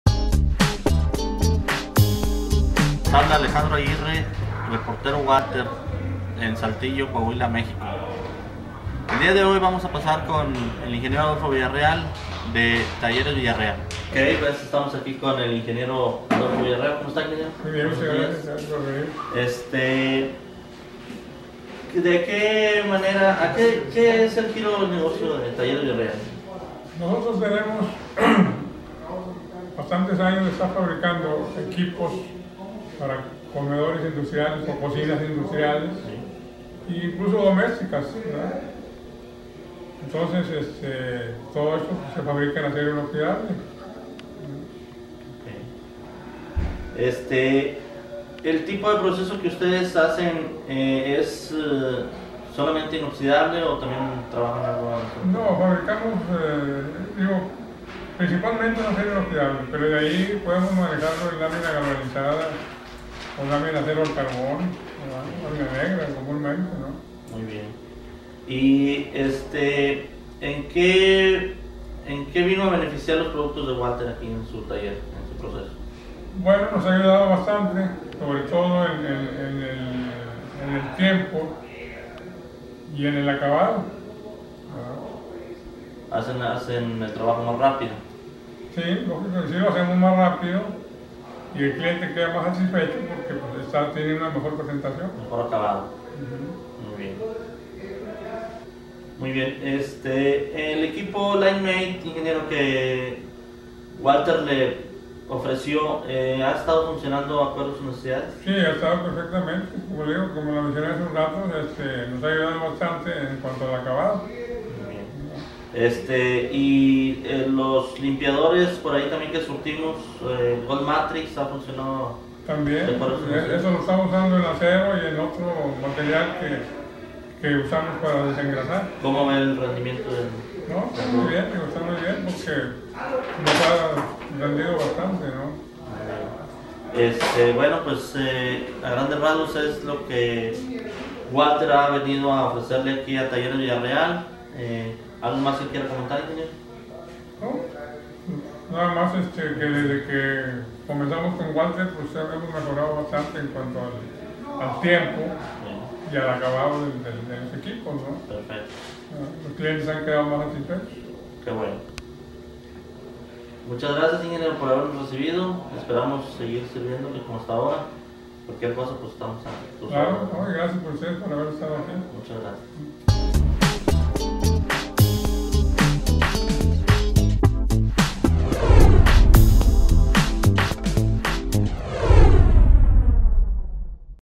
Talleres Villareal (Mexico)
Talleres Villareal improves customer satisfaction through quality production materials and tools for Stainless Steel fabrication.